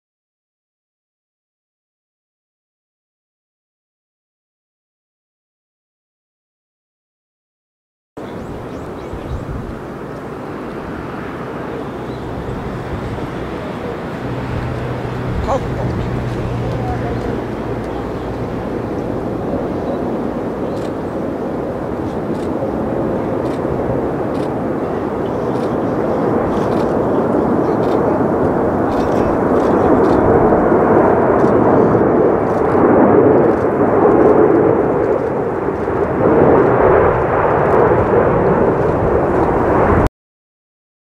Nippon Cargo Airlines (NCA).Boeing 747-8 Freighter (B747-8KZF/SCD) - JA12KZ (cn 36137/1422)
Nippon Cargo Airlines (NCA).
Boeing 747-8 Freighter (B747-8KZF/SCD)
Aircraft registration: JA12KZ (cn 36137/1422)
Landing approach.at Narita International Airport (NRT/RJAA) "Tokyo Narita Airport" JAPAN.
December 16,2013

日本貨物航空、ボーイング ７４７－８Ｆ形機（形式：Ｂ７４７－８ＫＺＦ/ＳＣＤ） 機体番号：ＪＡ１２ＫＺ（製造番号：３６１３７/１４２２）が着陸進入。
成田国際空港　２０１３年１２月１６日